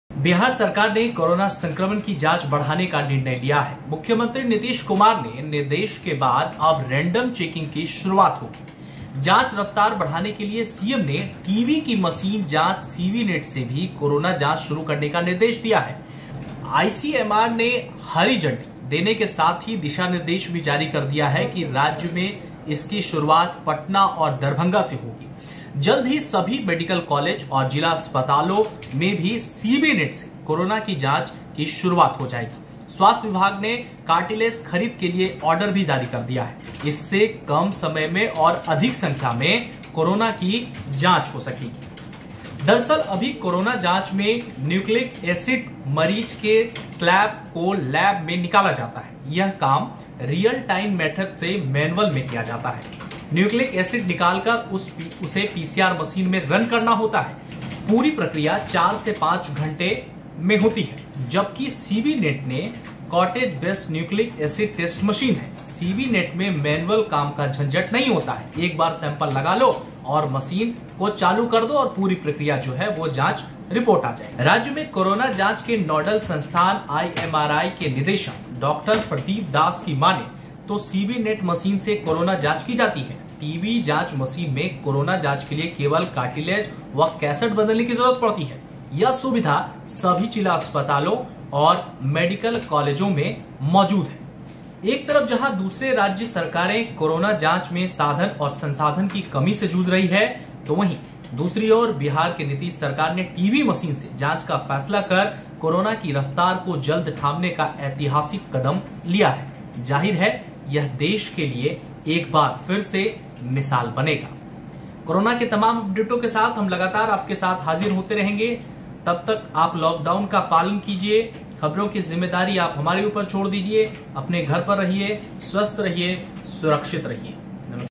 Nitish कुमार ने डॉक्टरों को बताया कैसे आएगी Corona जांच में तेजी
User: